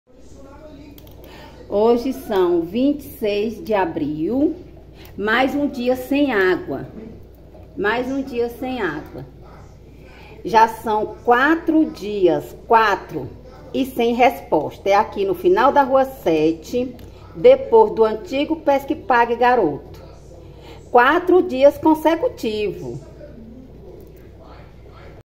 Setor da rua sete está sem fornecimento de água há quatro dias
O Portal jc24horas recebeu um vídeo enviado por uma colaboradora informando que os moradores da rua Sete de Setembro, na parte final  que fica no bairro São Cristóvão,  estão sem o fornecimento de água há quatro dias. Adriana de Sales gravou um vídeo ligando a torneira e mostrando a falta do líquido precioso. Durante a gravação ela informa  a data de hoje e diz que há quatro dias estão sem o fornecimento. Confira o vídeo.